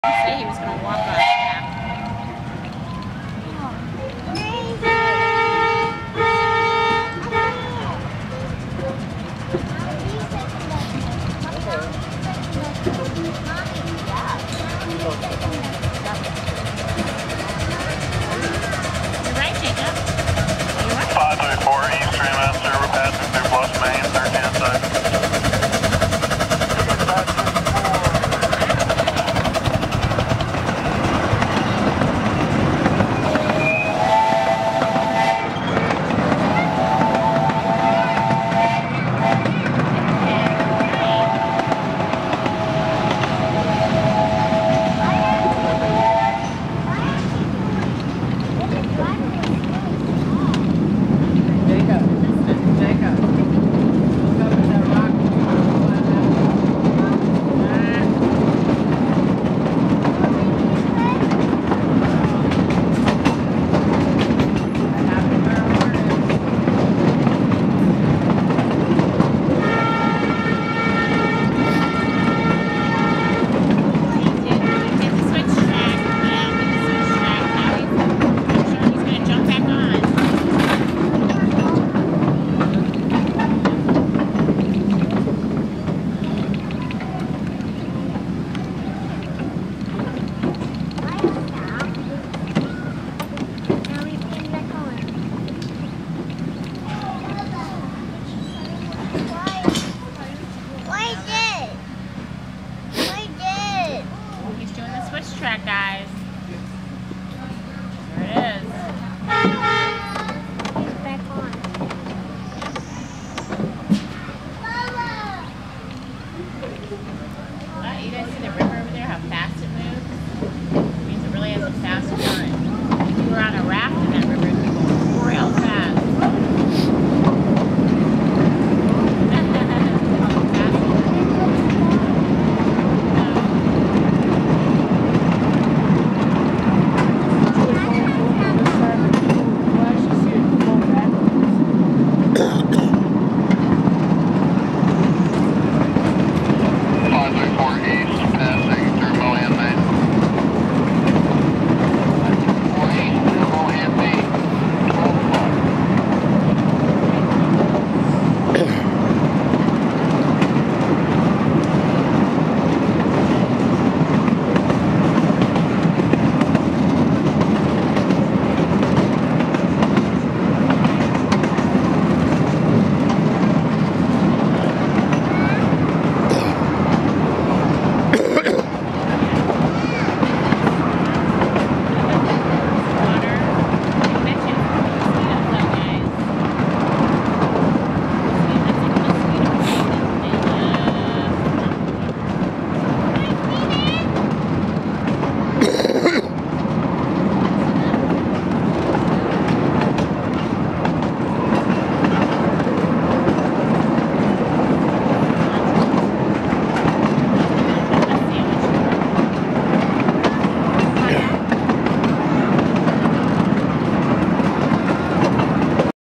Meet at Bluffs
13 waits in the siding for the passing of 534 and then heads to Glenco